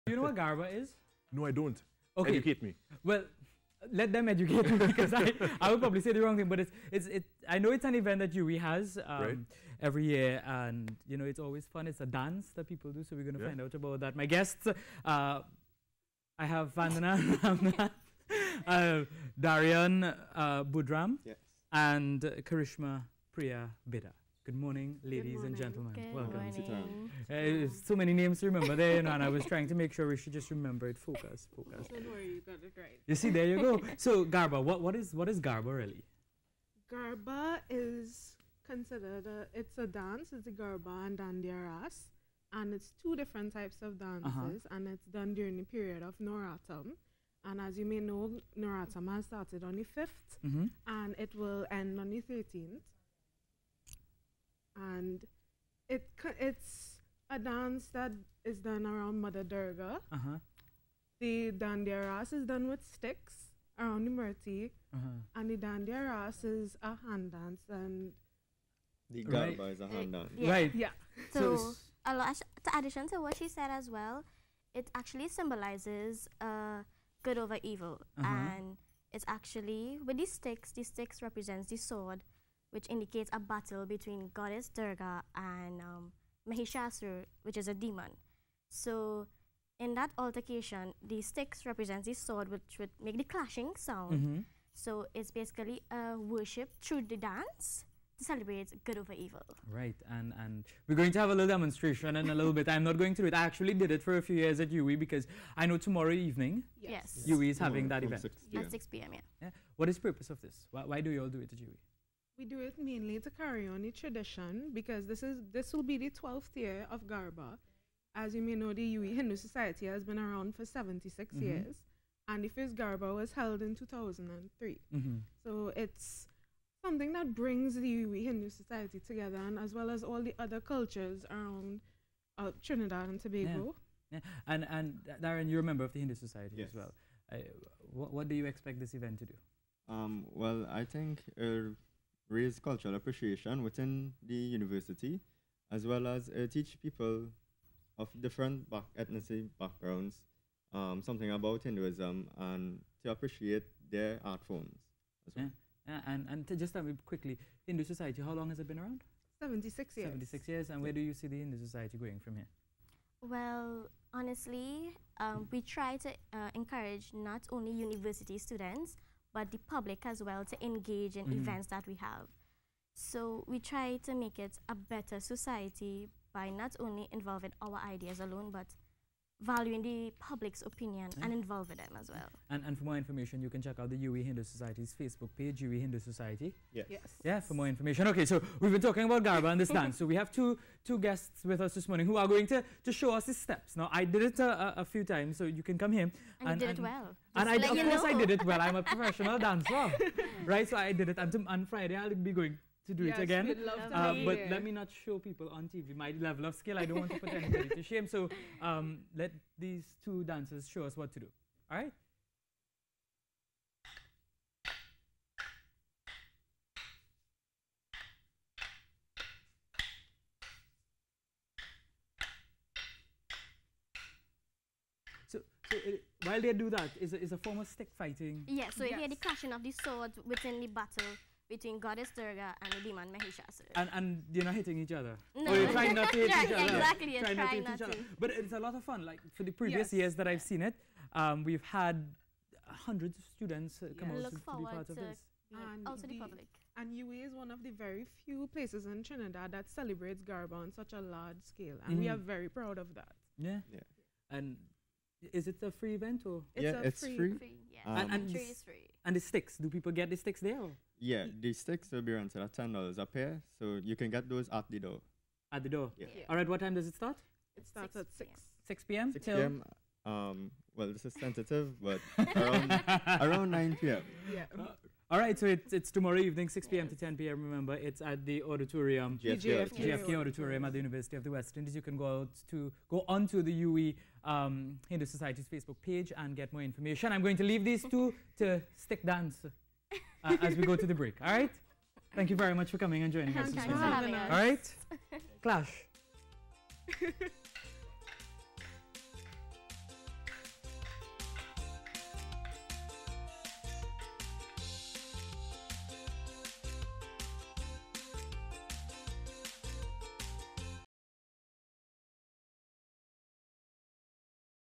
Garba Dancing
Members of the UWI Hindu Society - St. Augustine explain the types of dancing behind tomorrow's Garba and Dandiya Raas 2019.